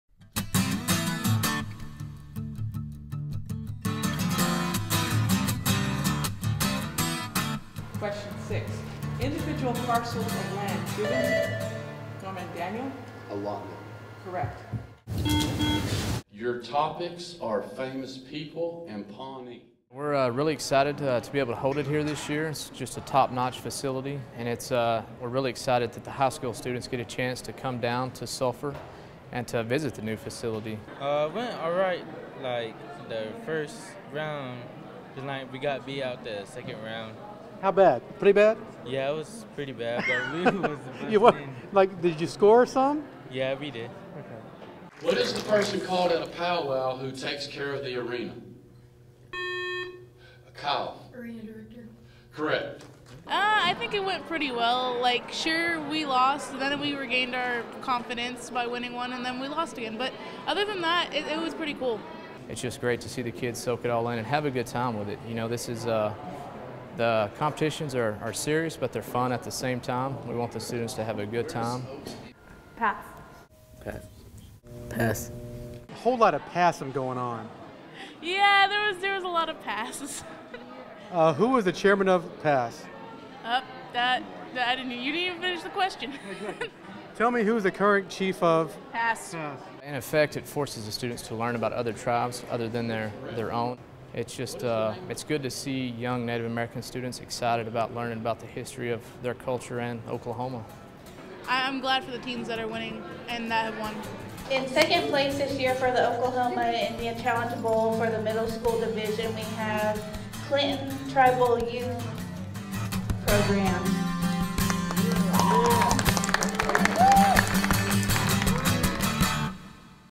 Community 046- Challenge Bowl 2014
Cheyenne and Arapaho TV (CATV 47) visited the 2014 Indian Challenge Bowl held at the Chickasaw Cultural Center in Slulphur, OK.  The Oklahoma Indian Challenge Bowl is an academic competition for Oklahoma
Indian students enrolled in grades 7th -- 12th.The Oklahoma Indian Challenge Bowl is designed to stimulate Indian Students, their parents and adult sponsors to learn about tribal people, accurate tribal histories,
cultural educations and emphasize the importance of tribal languages that make the
39 Indian tribes located in Oklahoma unique.